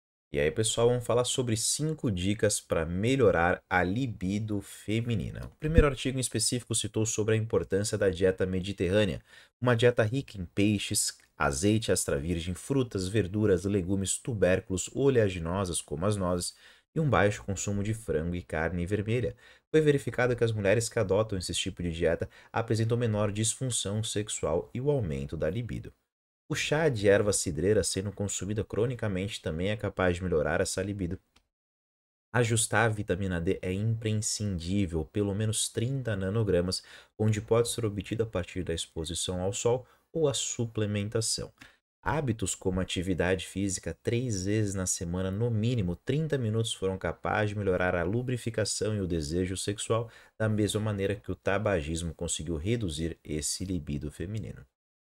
05 dicas para aumentar a Libido feminina
- Dieta do Mediterrâneo: Rica em frutas, nozes, legumes, cereais e peixes. O azeite de oliva é a principal fonte de gordura e com ingestão baixa a moderada de vinho, bem como baixo consumo de carne vermelha e aves. No presente estudo, as mulheres com maior adesão à dieta mediterrânea tiveram a menor prevalência de disfunção sexual

- Chá de Erva-Cidreira: Aumento do desejo, excitação e redução da dor sexual

- Vitamina D: A suplementação de vitamina D melhora o funcionamento sexual feminino e o humor em mulheres com baixo nível de vitamina D

- Atividade física: Mulheres com disfunção sexual diagnosticável, ao iniciar um regime de 30 minutos de exercícios vigorosos 3x por semana, foi suficiente para produzir melhorias clinicamente relevantes na função sexual, particularmente no desejo sexual

- Tabagismo: Alguns estudos mostram que fumar era um fator de risco independente de disfunção sexual feminina. A causa pode ser devido ao fato de afetar as respostas de excitação

Referência:
Lorenz, T. et al "Exercise improves sexual function in women taking antidepressants: Results from a randomized crossover trial" Depress Anxie, 2015
Choi, J et al "Dose-response relationship between cigarette smoking and female sexual dysfunction", Obstet Gyn, 2015
Kashani, Z. et al "Effect of Melissa officinalis (Lemon balm) on Sexual Dysfunction in Women: A Double- blind, Randomized, Placebo-controlled Study" Iran J, 2018
Giugliano, F et al "Adherence to Mediterranean diet and sexual function in women with type 2 diabetes" J Sex Med, 2010
Krysiak, R et al "The effect of vitamin D supplementation on sexual functioning and depressive symptoms in young women with low vitamin D status" Endo Pol, 2018
- Atendimento Presencial em Rua Cotoxó, 611 cj 74 – Perdizes – São Paulo/SP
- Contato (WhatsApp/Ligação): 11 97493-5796

Respeite sua individualidade biológica. Nutrição inteligente, sempre!